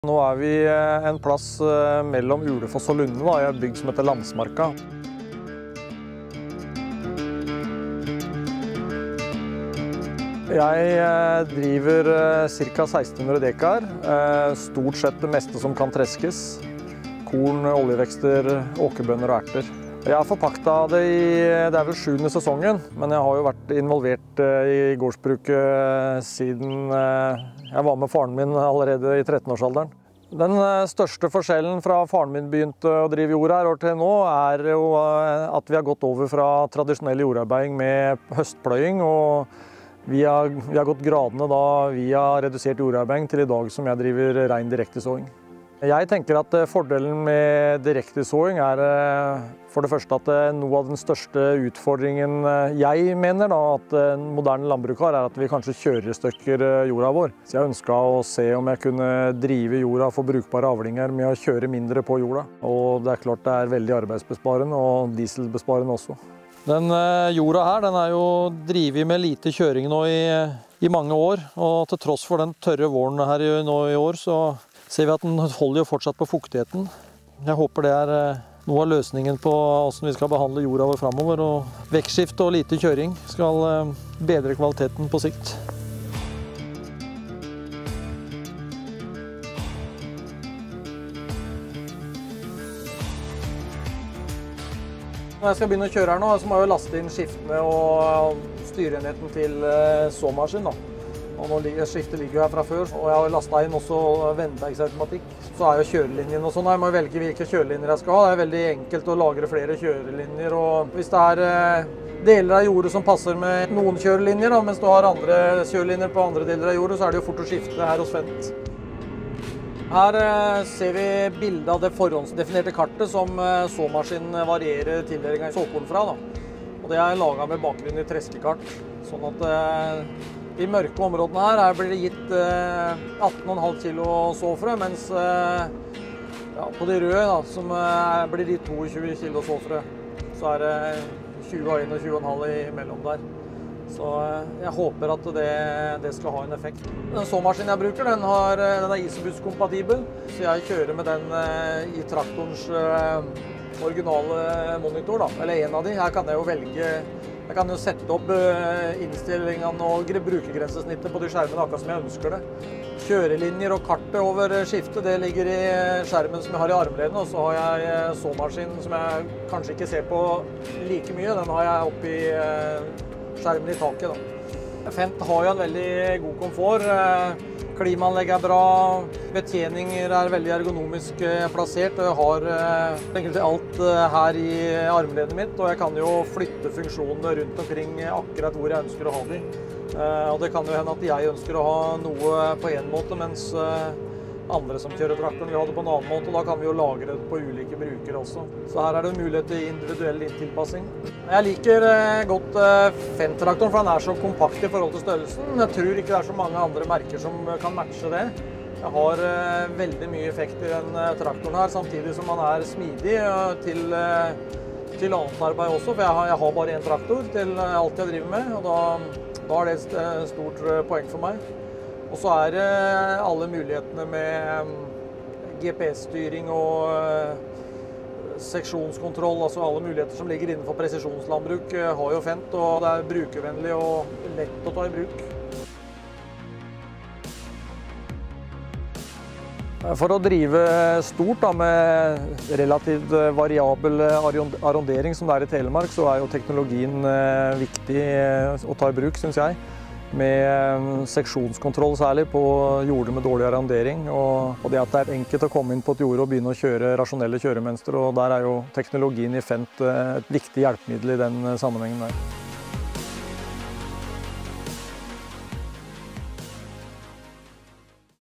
Teknologi for det norske landbruket | Fendt 700-serie
I Landsmarka i Telemark møter vi Henrik Kjeldsen som driver 1600 mål på Holla gård. Gården har han forpaktet i sju sesonger, men har vært inkludert i drifta helt siden 13-års alderen, da hans far forpaktet den. På disse årene, er det mye som har endret seg. Les mer